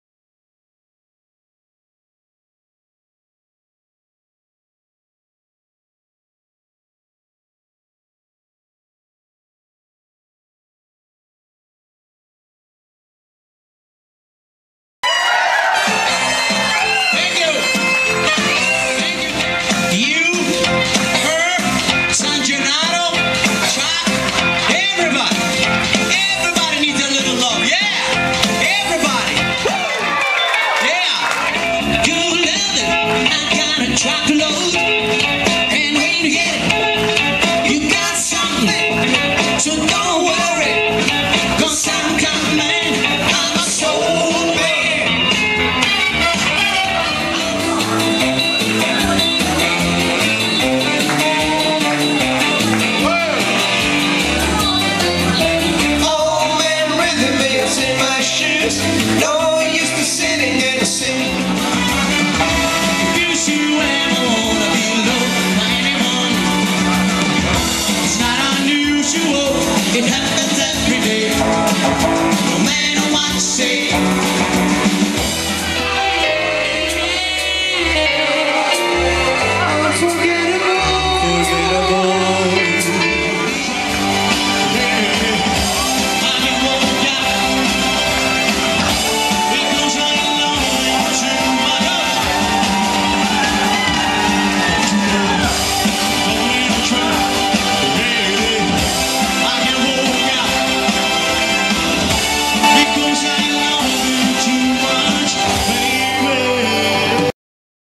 Happy Days Band from Upstate New York
Video clips from Sun Key Village "Senior" Prom on 2/28/09